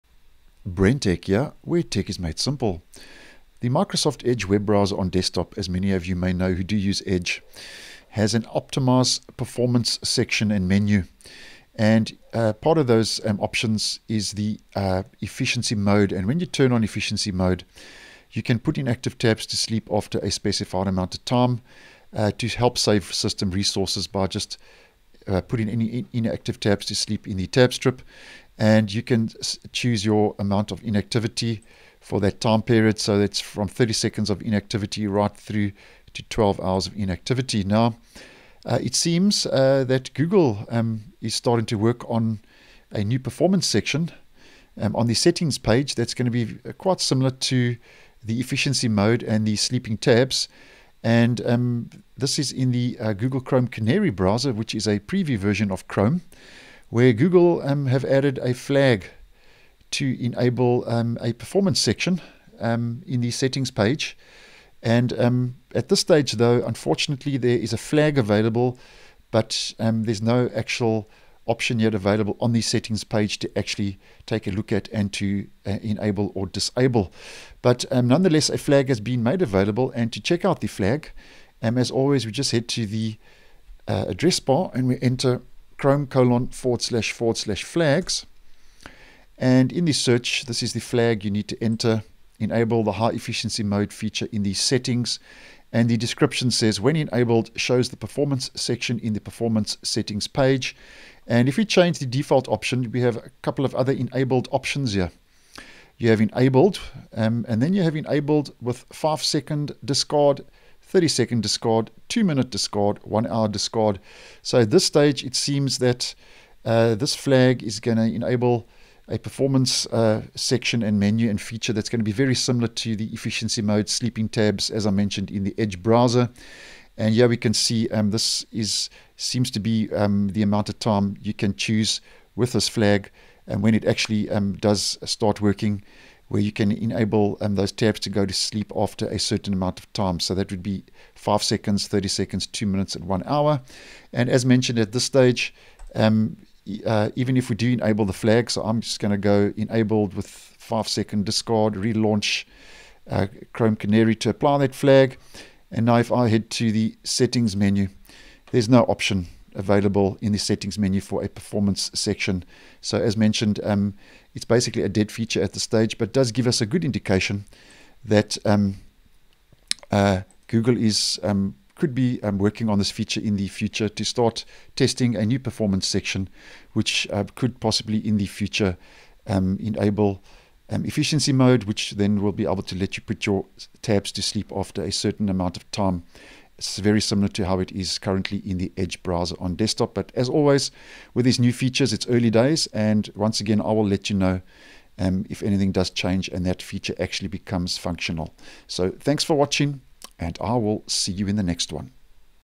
Google could be bringing a new Performance section with Efficiency mode to Settings in Chrome
As spotted by u/Leopeva64-2 on Reddit, in this section you will be able to configure a 'high efficiency mode' and a 'battery saving mode' that will discard inactive tabs in the background after the specified amount of time. Google has already added the flag in Chrome Canary to enable this feature, but the Performance section is not yet available on the Settings page.